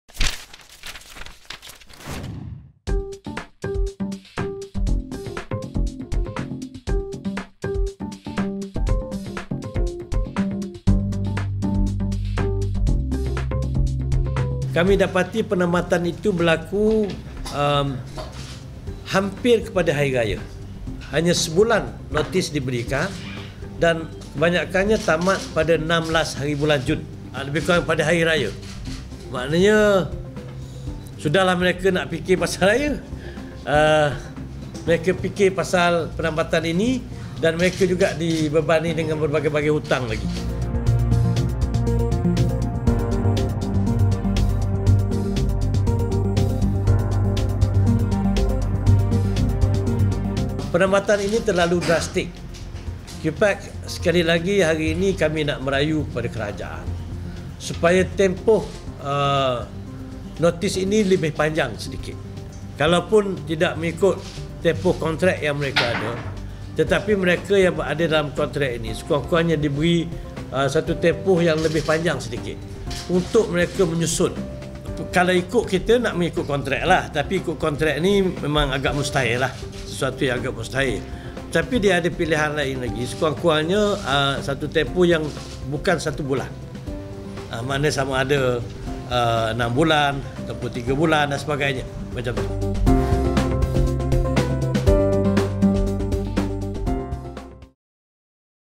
NEWS: Cuepacs: Delay termination of civil servants
Cuepacs is urging the government to delay the termination of non-political civil servants.